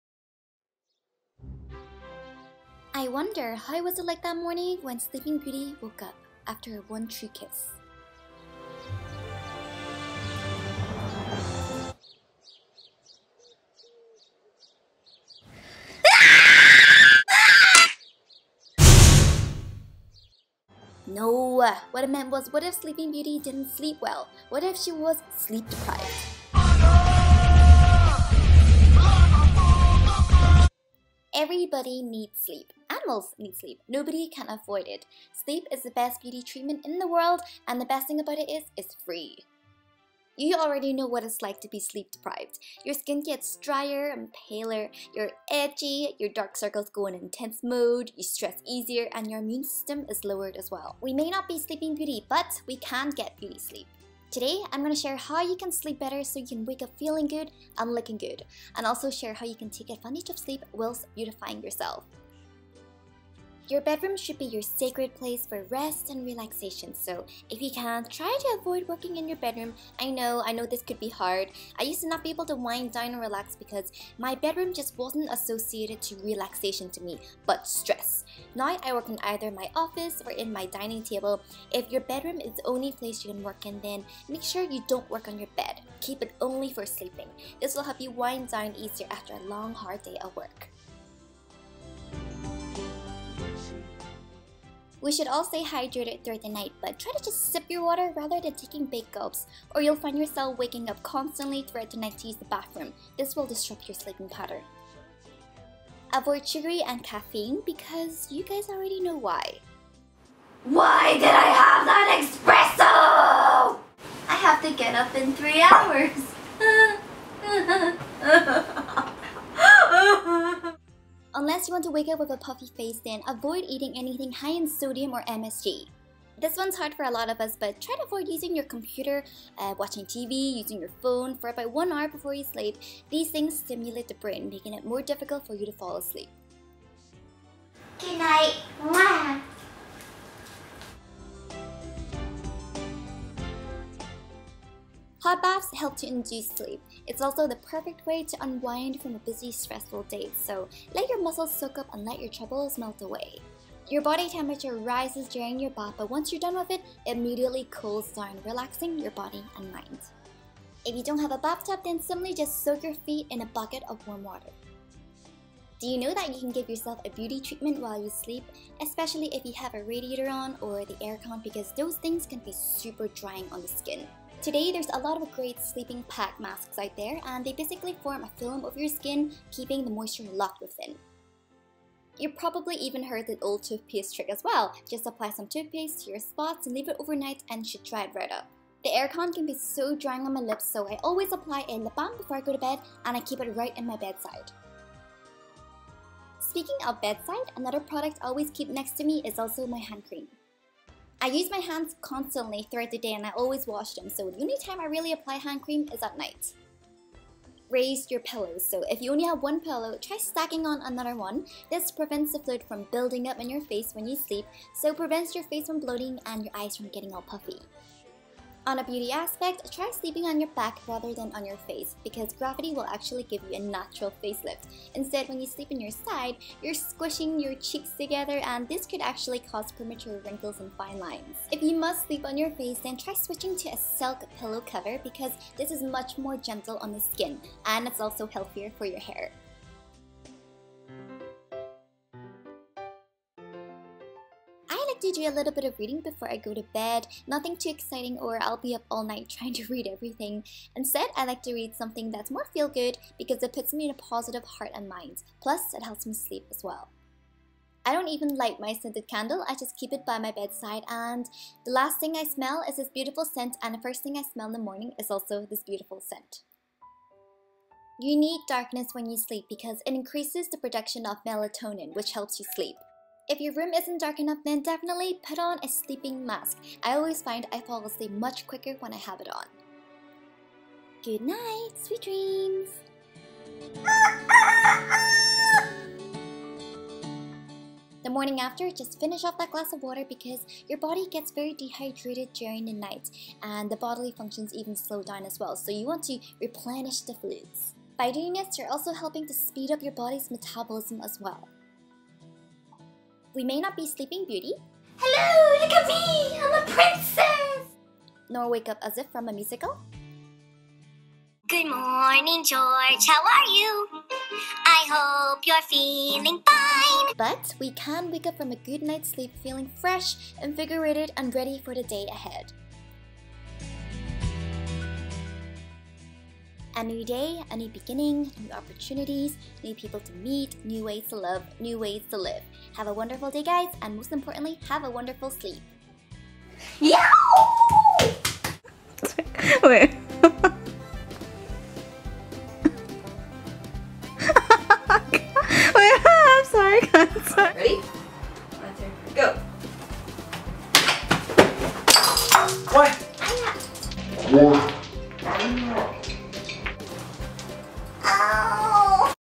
Sleeping Beauty?
Hello my sleeping beauties!

What if Sleeping Beauty didn't sleep well at all? What if she went into Incredible Hulk mode? That would make an awesome movie right?

We all need sleep. Nobody can avoid it.  When we sleep, our body regenerates itself so we can be recharge for the day ahead of us. It's the best beauty treatment in the world and best thing about it is, it's free.

Ok so we all already know how to sleep so no need to share how to do it (close eyes).

In this video, I share how you can sleep better so you can wake up refreshed and invigorated for the day ahead. I'll also share ways you can beautify yourself during your sleep too.

Thank you Tim for being the erm... Prince that gave me the one true kiss. However, why was that lamp more important than me huh???  Hmph!

Ps. RIP lamp. Thank you for revenging by dropping a crystal sphere on my head before you died.

Pps. Veggie Tales for the win.

Ppps. Just a little heads up! We are closing our online store for 3 weeks for the wedding! Hope you guys understand =)

Much love, Bubz xx